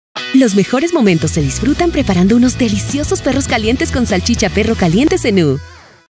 ¡Disfruta tus mejores momentos con unos deliciosos Perros Calientes! | Zenú
Los momentos que pasas en familia o con amigos serán mucho mejores con la compañía de un delicioso Perro Caliente con Salchicha Perro Caliente de Zenú.

Únete a nuestra comunidad digital para descubrir más momentos Zenú​​